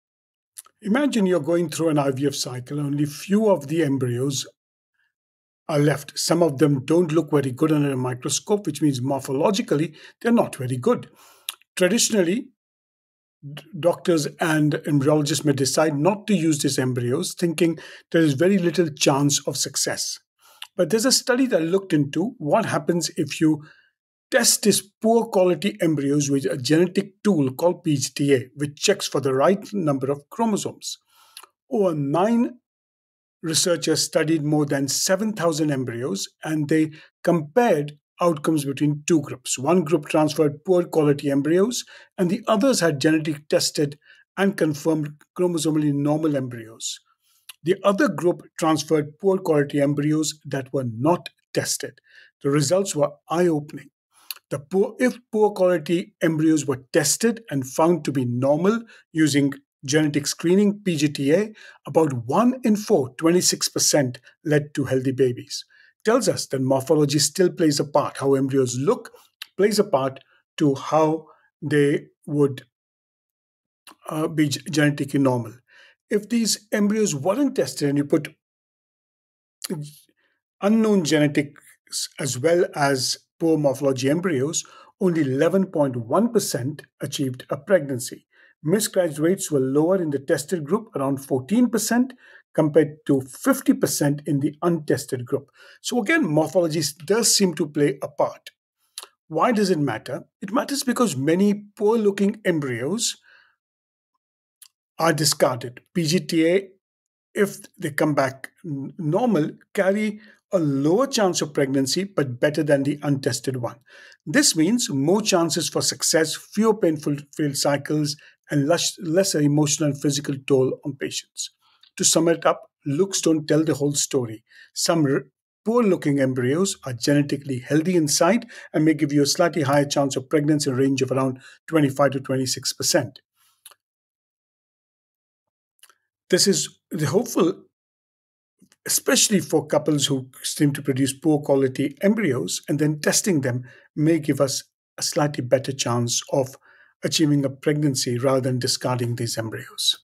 Camera Recording - Oct 28, 2025-VEED
Ever wonder if the not-so-perfect embryo could bring you a perfect little one? 🌱 New research finds nearly 1 in 4 “poor-quality” embryos, when genetically tested, lead to healthy births—more than double the rate of untested ones. Miscarriage risk also plummets. For many, this means hope, fewer treatments, and emotional relief. Could your “maybe” embryo be a “yes”? 💫
l Hashimi B, Harvey KE, Harvey SC, Linara-Demakakou E, Griffin DK, Ahuja K, Macklon N (2025). “PGT-A can increase the number of embryos available for transfer by ‘rescuing’ morphologically poor-quality blastocysts: an analysis of nine years of data from a single centre.” Reproductive Biomedicine Online. doi:10.1016/j.rbmo.2025.105208.